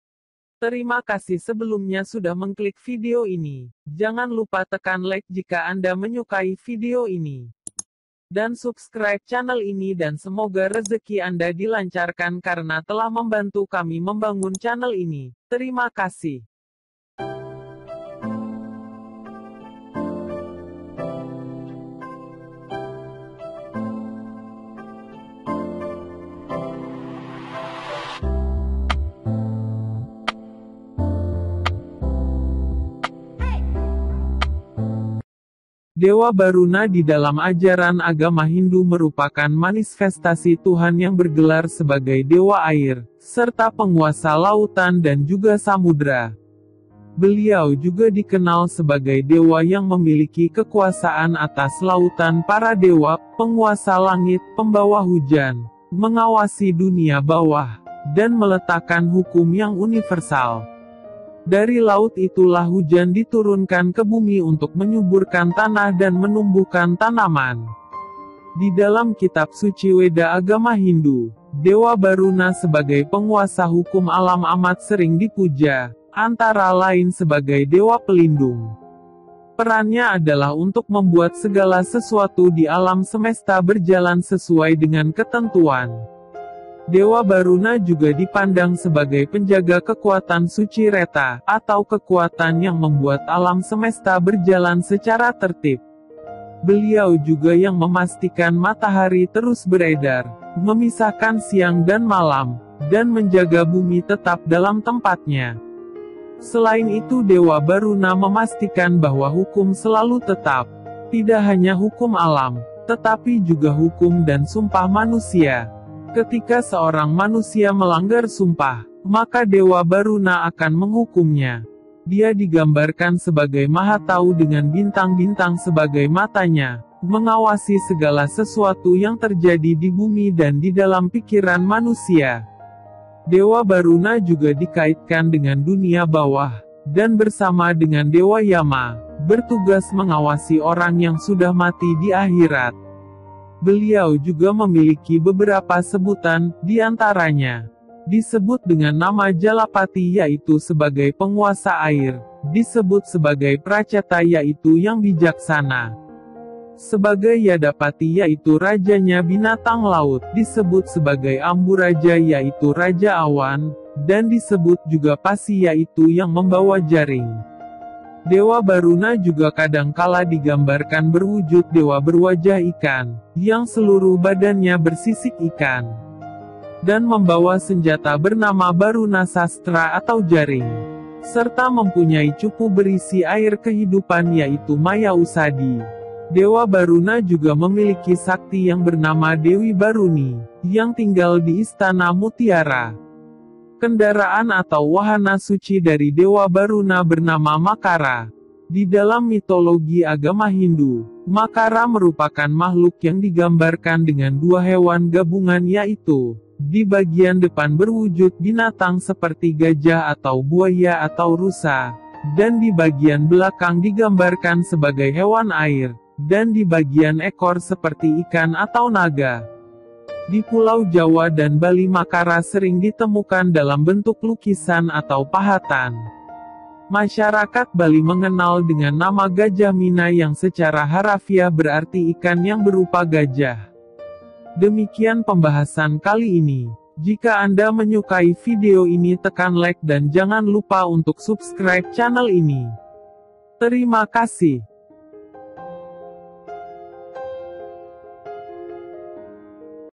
Baruna Sang Penguasa Samudra
Dewa BARUNA di dalam ajaran agama hindu merupakan manifestasi Tuhan yang bergelar sebagai dewa air, serta Penguasa lautan dan juga Samudra. Beliau juga dikenal sebagai dewa yang memiliki kekuasaan atas lautan para dewa , penguasa langit, pembawa hujan, mengawasi dunia bawah, dan meletakkan hukum yang universal. Dari laut itulah hujan diturunkan ke bumi untuk menyuburkan tanah dan menumbuhkan tanaman.